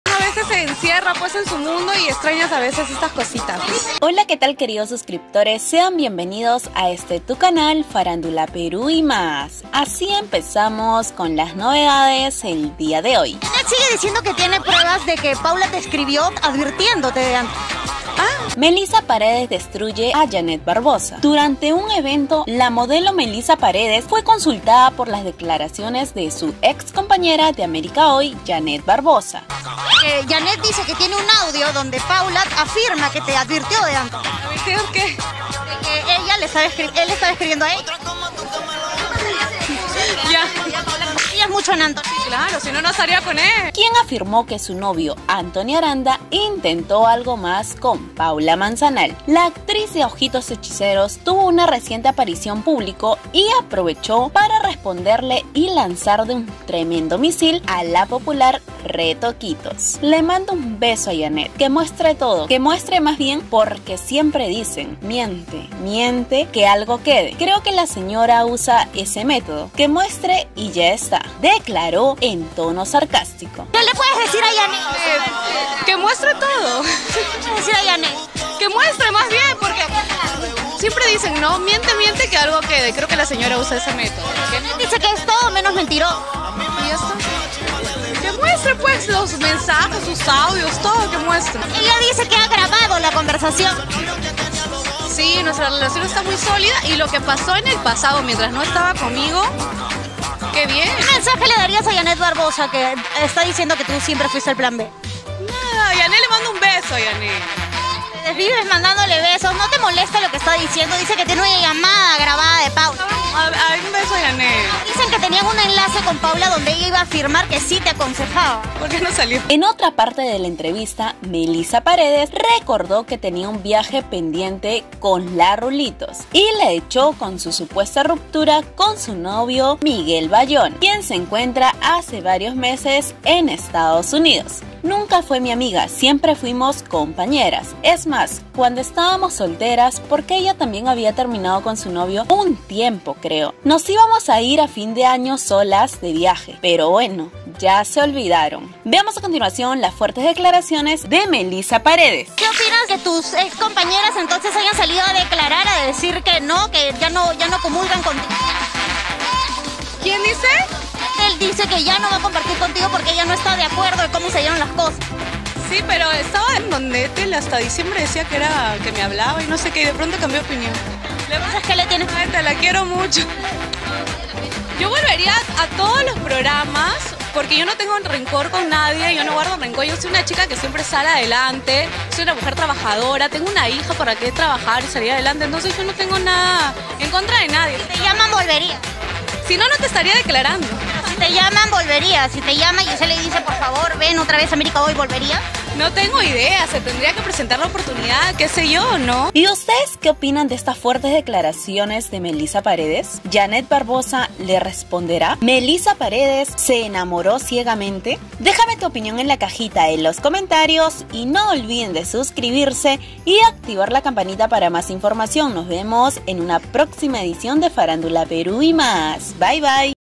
EN SHOCK!! MELISSA PAREDES NO SE CAYO NADA Y D3STRUY3 A SUS EX'S COMPAÑERAS DE TRABAJO Y DETRACTORES
ASI SE EXPRESO MELISSA PAREDES DESPUES DE RECIBIR TANTA CRITICA POR PARTE DE JANET BARBOZA Y ETHEL POZO FUERTE Y CLARO DECLARO LA EX ESPOSA DE RODRIGO EL GATO CUBA.